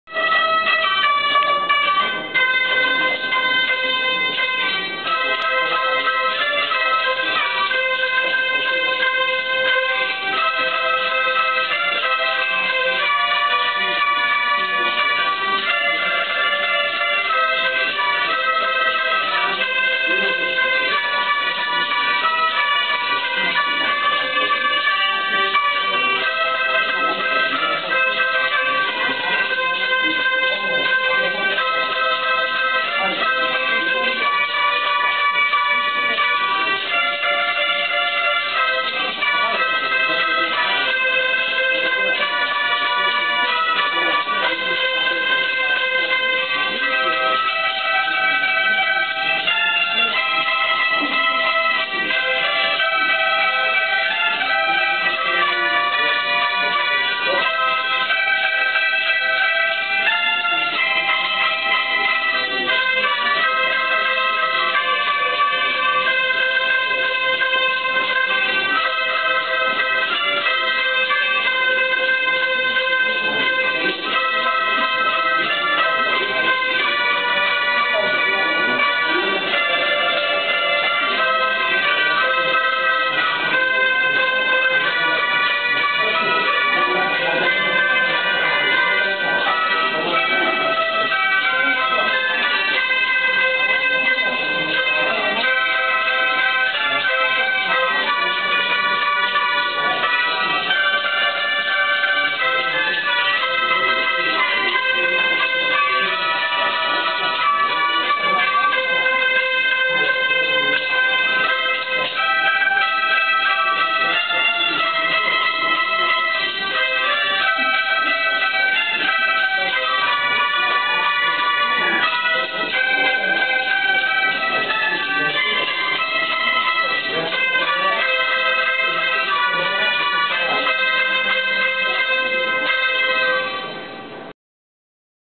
T.J.C. Taiwan 東南區傳道靈修會 - 鹽濱教會中山琴演奏
T.J.C. Taiwan 東南區傳道靈修會 101年8月於嘉平教會 鹽濱教會中山琴演奏 1